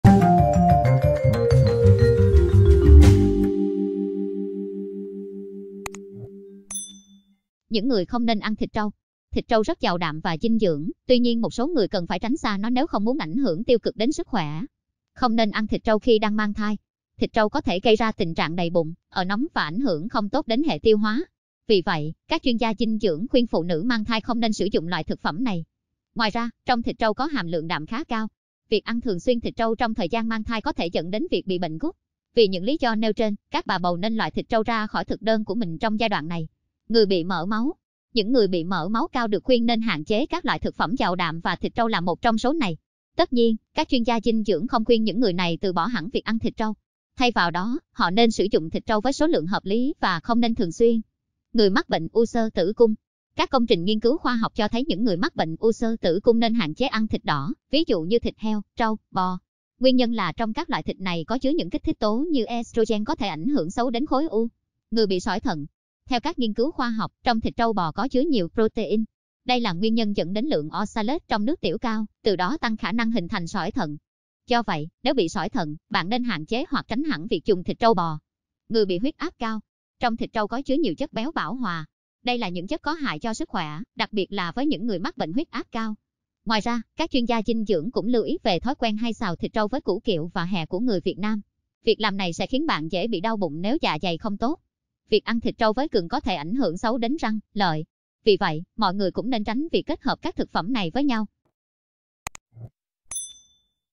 Những người không nên ăn thịt trâu - Kênh Hướng Dẫn
Thịt trâu rất giàu đạm và dinh dưỡng, tuy nhiên một số người cần phải "tránh xa" nó nếu không muốn ảnh hưởng tiêu cực đến sức khỏe.